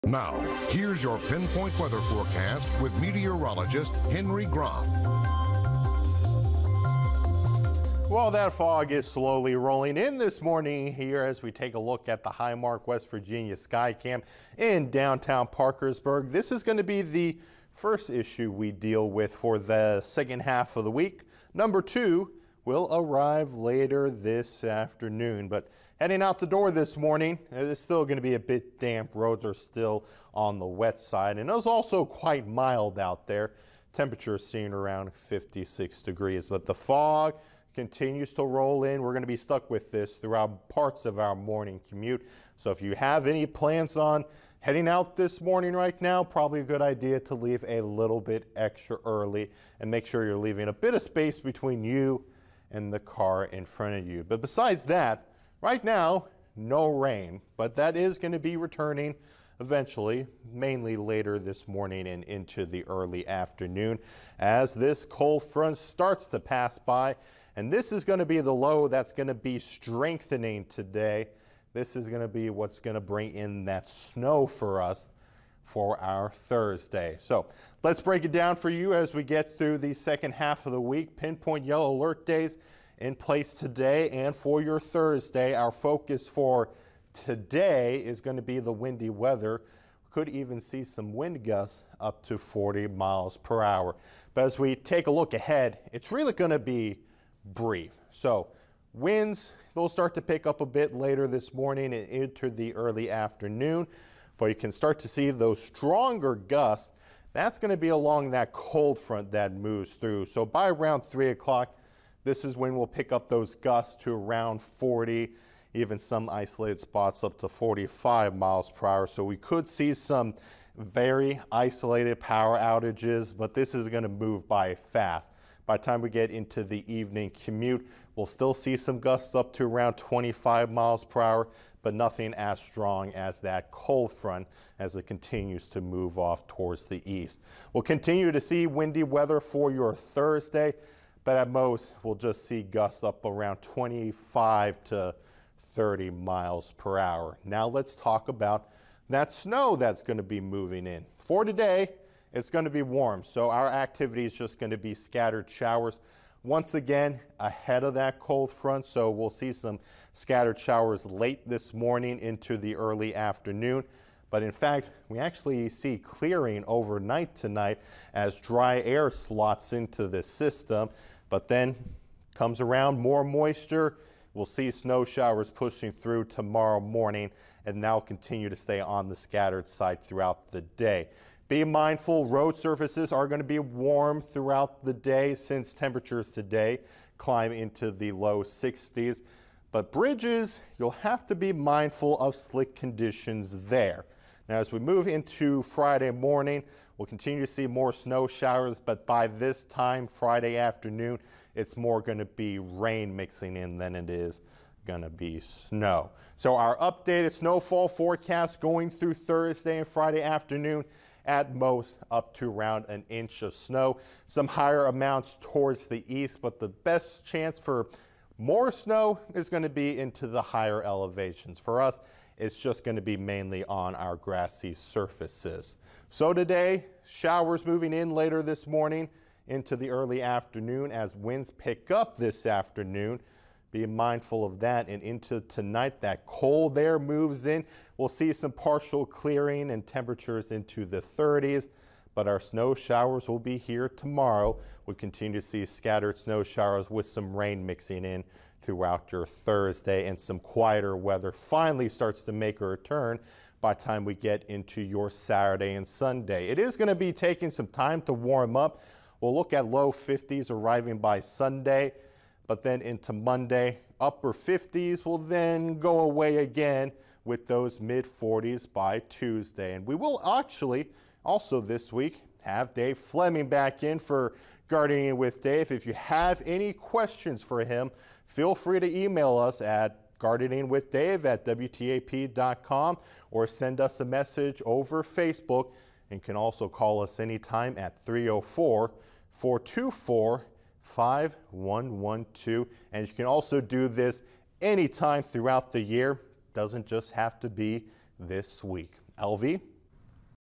WTAP Pinpoint Weather AM Update 11/20
Pinpoint Yellow Alert Day | Windy weather arrives Wednesday afternoon ahead of the chance of snow on Thursday.